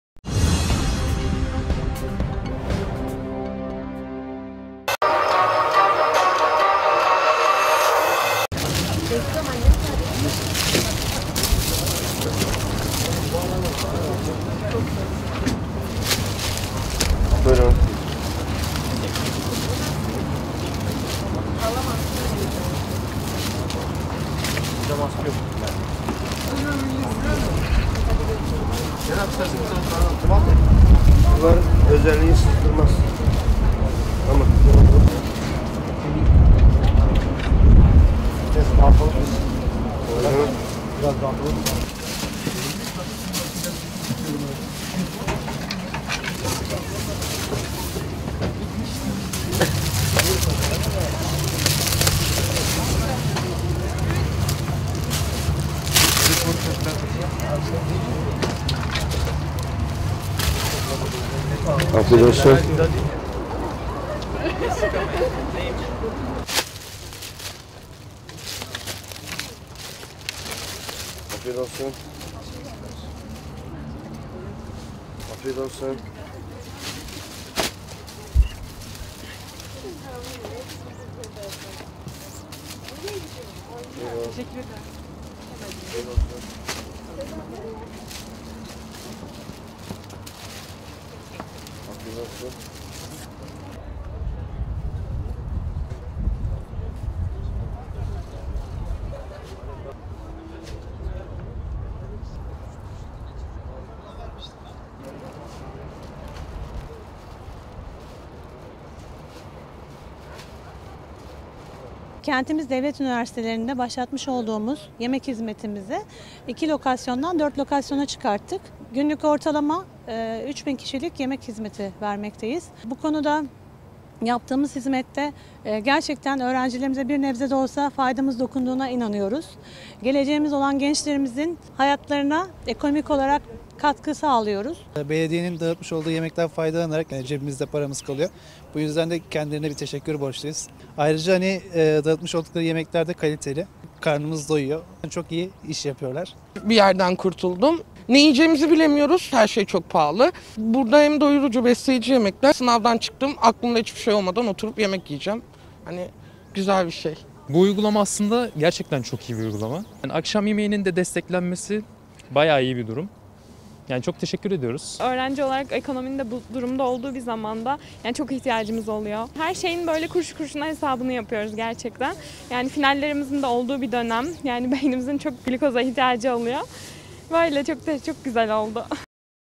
Sıcak yemek hizmeti Kâtip Çelebi Üniversitesi’ndede başladı
İzmir Büyükşehir Belediyesi, Dokuz Eylül Üniversitesi, Ege Üniversitesi ve
İzmir Yüksek Teknoloji Enstitüsü’nün ardından Kâtip Çelebi Üniversitesi’nde
de öğrencilere sıcak yemek hizmetini başlattı. Giderek ağırlaşan yaşam koşulları
nedeniyle her şeyi kuruşu kuruşuna hesaplamak zorunda kaldıklarını söyleyen
öğrenciler, uygulama için Başkan Tunç Soyer’e teşekkür etti.
İzmir Büyükşehir Belediye Başkanı Tunç Soyer’in eğitimde fırsat eşitliği ilkesi doğrultusunda
üniversite öğrencilerine verilen destek artarak sürüyor. Ege Üniversitesi, Dokuz Eylül
Üniversitesi (DEÜ) ve İzmir Yüksek Teknoloji Enstitüsü’nün (İYTE) ardından artık Kâtip
Çelebi Üniversitesi öğrencileri de İzmir Büyükşehir Belediyesi’nin dağıttığı sıcak yemeklere
ulaşabiliyor. Aşevleri Şube Müdürlüğü, Kâtip Çelebi Üniversitesi Çiğli Kampüsü içindeki
kütüphanenin önünde hafta içi her gün 17.00 ile 19.00 saatleri arasında yemek dağıtmaya
başladı. Giderek ağırlaşan yaşam koşulları nedeniyle geçinmekte zorlanan öğrenciler
uygulamadan memnun.
Dört noktada üç bin kişilik yemek
Aşevleri Şube Müdürü Ebru Asaldevlet üniversitelerinde başlatılan yemek hizmetini ikiden
dört noktaya çıkardıklarını söyledi. Bu dört noktada günlük ortalama üç bin kişilik sıcak
yemek dağıtıldığını ifade eden Asal, “Bu hizmetle onlara bir nebze de olsa faydamızın
dokunduğuna inanıyoruz. Geleceğimiz olan gençlerimiz için ne yapsak az” diye konuştu.
“Hem paramız cebimizde kalıyor hem de doyuyoruz”
Üniversite öğrencilerinden Yasin Günlü, “Bu uygulama ile paramız cebimizde kalıyor. Bu
nedenle Büyükşehir Belediyesi’ne teşekkür borçluyuz. Ayrıca dağıtılan yemekler kaliteli.
Karnımız doyuyor. Çok iyi iş yapıyorlar” dedi.
“Yemek maliyetinden kurtulduk”
Şenol Çavdar henüz ayın ortası olmasına rağmen maddi olarak zorlanmaya başladığını
belirterek, “Yemek maliyetinden kısmen kurtulduk. Ayrıca ne yiyeceğimizi de bilemiyorduk.
Her şey çok pahalı. Buradaki yemekler besleyici, doyurucu ve ücretsiz. Şimdi sınavdan çıktım
ve oturup burada yemeğimi yiyeceğim. Güzel bir uygulama” şeklinde konuştu.
“Her şeyin kuruşu kuruşuna hesabını yapıyoruz”
Furkan Doğru da öğrencilere yönelik böyle bir uygulama olmasının çok iyi olduğunu
söyleyerek “Akşam yemeğinin buradan karşılanması bizim için ekstra bir yardım. İzmir
Büyükşehir Belediyesi’ne çok teşekkür ediyoruz” dedi.
Ezgi Çetin ise “Ekonominin bu durumda olduğu günlerde, öğrenciler olarak bu desteğe
gerçekten ihtiyacımız oluyor. Kuruşu kuruşuna her şeyin hesabını yapıyoruz. Finallerin
olduğu bir dönemindeyiz. Beynimizin glikoza ihtiyacı var. O nedenle bu uygulama çok güzel
oldu” ifadelerini kullandı.
Sabahları altı noktada çorba hizmeti

Dört noktada hafta içi her gün 17.00 ile 19.00 saatleri arasında öğrencilere yemek dağıtan
İzmir Büyükşehir Belediyesi’nin altı noktada oluşturduğu Çorba Durakları da öğrencilere
sıcak çorba hizmetini sürdürüyor. Dokuz Eylül Üniversitesi (DEÜ) Eğitim Fakültesi, DEÜ
İlahiyat Fakültesi, DEÜ İktisadi ve İdari Bilimler Fakültesi, Dokuz Eylül Üniversitesi (DEÜ)
Tınaztepe Yerleşkesi Girişi, Kâtip Çelebi Üniversitesi ile Ege Üniversitesi Kampüsü
yakınındaki Bornova Metro istasyonunda kurulan Çorba Durakları, hafta içi sabah 07.30 ile
09.00 saatleri arasında hizmet veriyor.